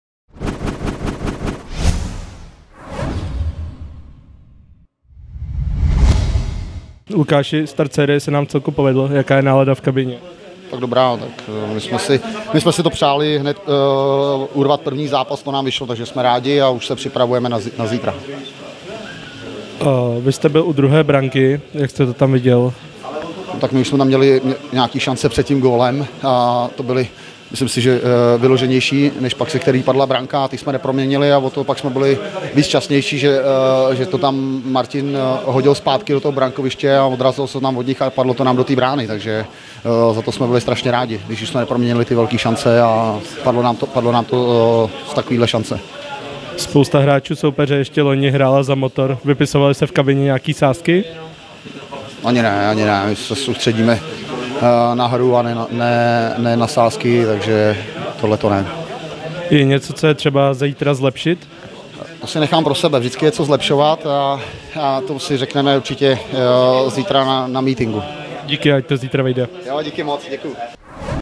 Rozhovor: VHK ROBE Vsetín - ČEZ Motor České Budějovice (1. SF)
Úspěšný start do semifinálové série zhodnotil Lukáš Endál, který byl u druhé branky.

Tazatel a střihač: Marek Baloun
Kameraman: Adam Pavlíček